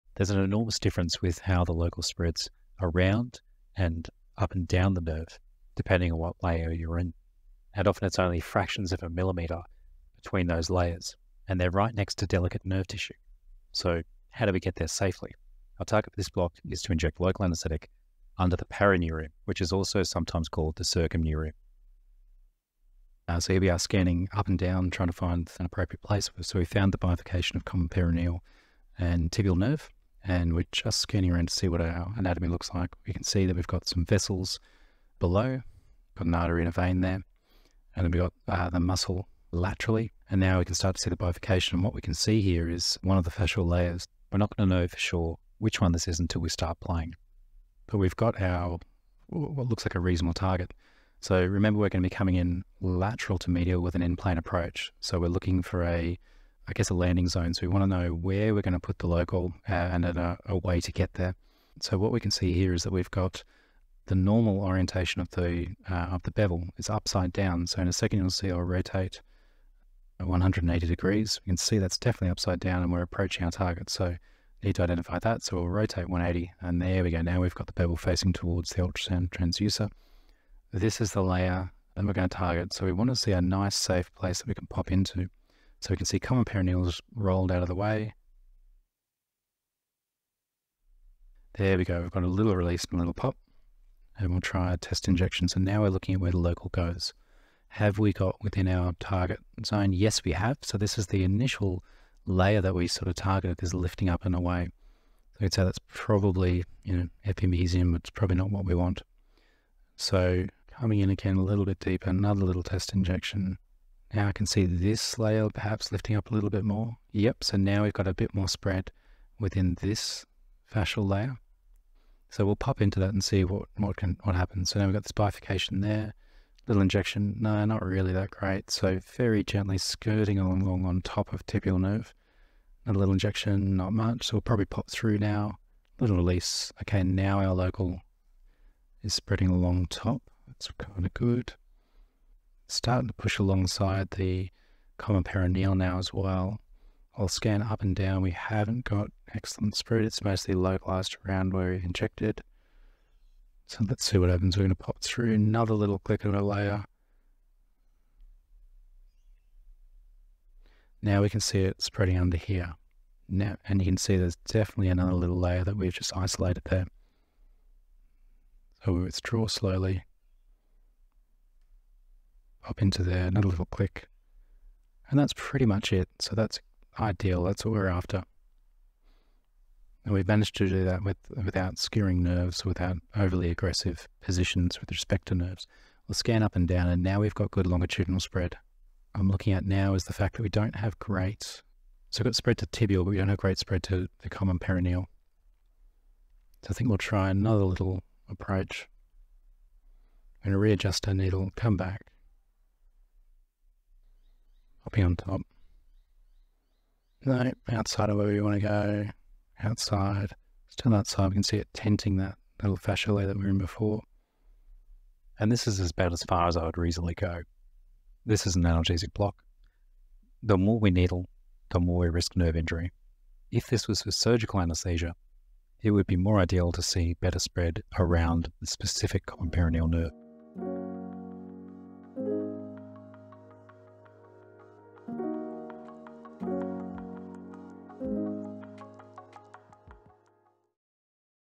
Popliteal Sciatic nerve block - get close safely
Popliteal sciatic blocks are a very commonly performed block.

It requires a reasonably close injection of local under the paraneurium (aka circumneurium) for improved success, speed of onset, and duration of effect.

There are a lot of "micro decisions" that occur when guiding the local to the correct spot in a safe manner.